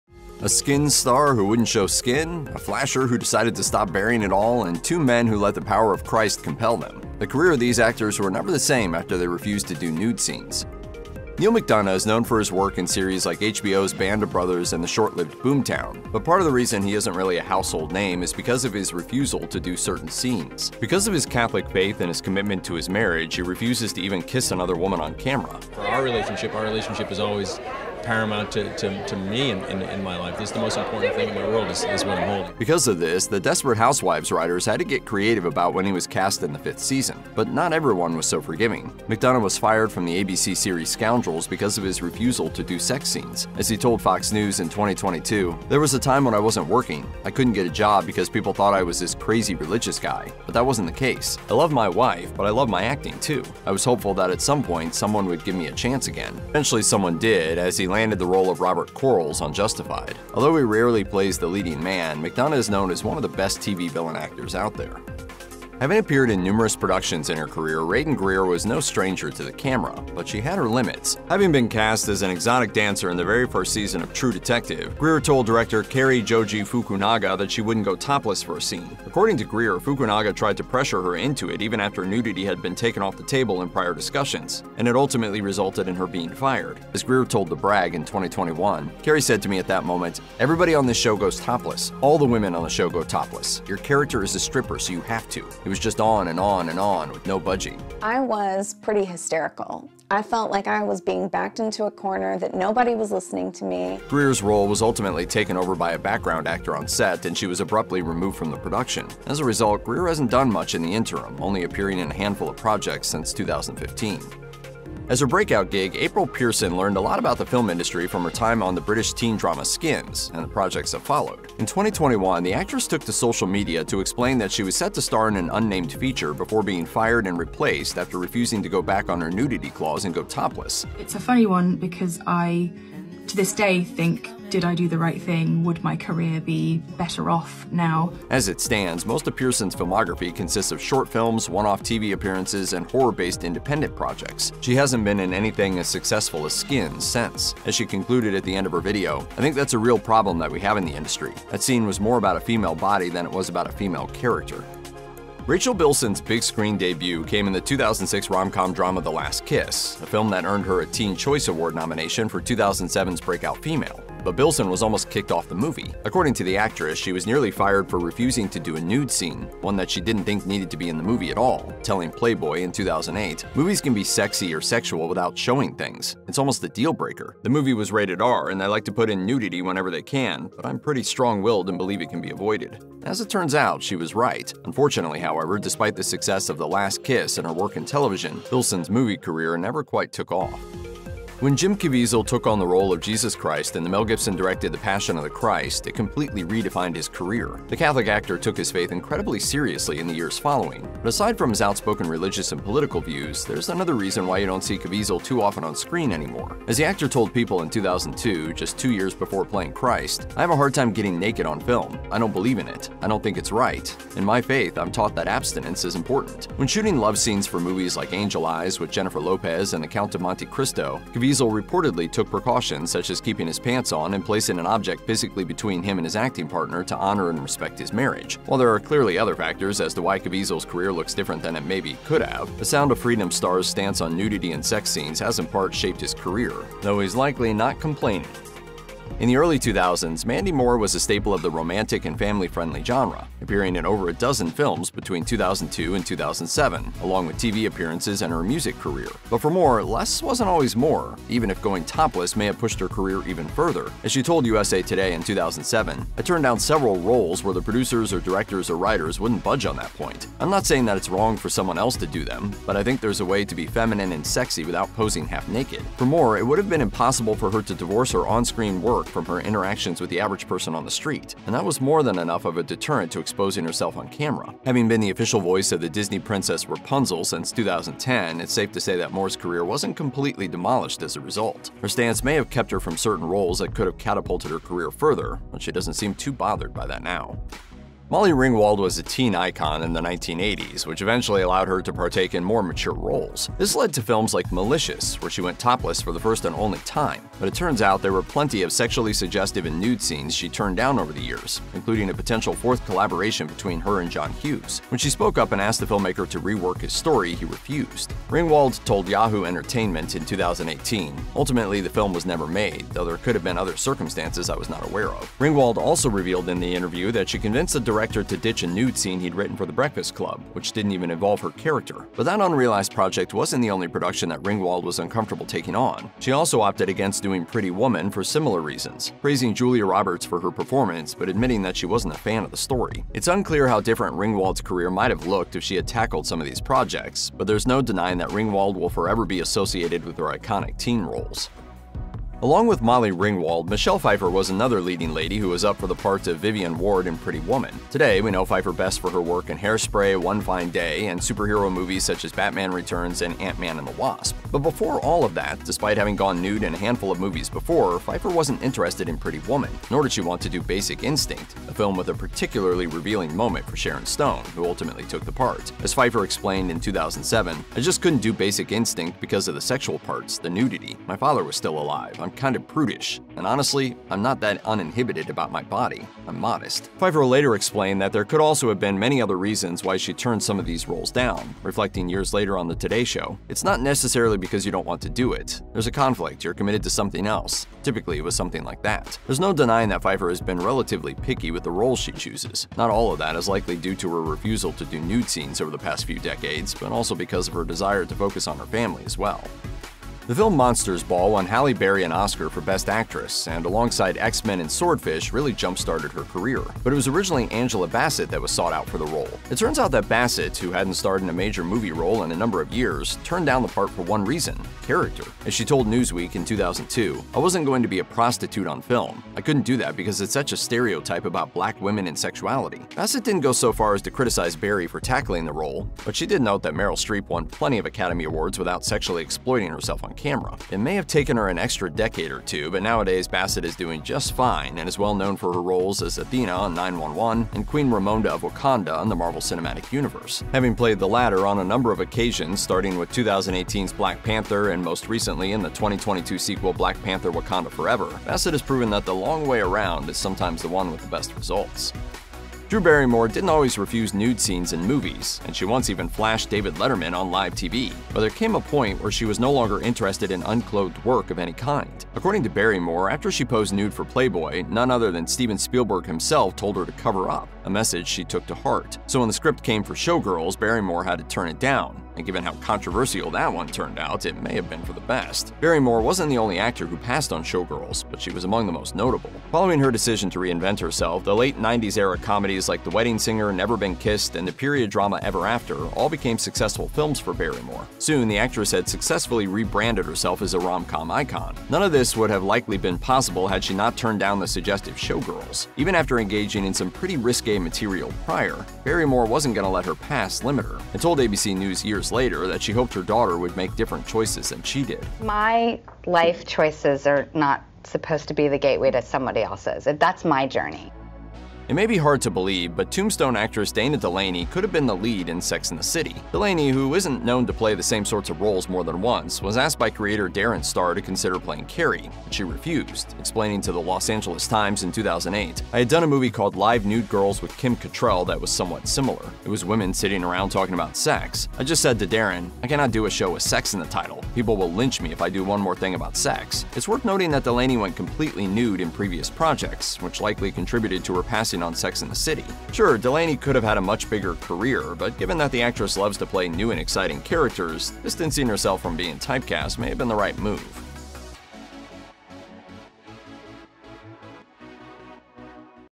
Actors Whose Careers Were Never The Same After Refusing Nude Scenes
A 'Skins' star who wouldn't show skin, a flasher who decided to stop baring it all, and two men who let the power of Christ compel them. The careers of these actors were never the same after they refused to do nude scenes.

Neal McDonough | 0:00
Raeden Greer | 1:05
April Pearson | 1:55
Rachel Bilson | 2:35
Jim Caviezel | 3:11
Mandy Moore | 3:59
Molly Ringwald | 4:48
Michelle Pfeiffer | 5:42
Angela Bassett | 6:40
Drew Barrymore | 7:32
Dana Delany | 8:31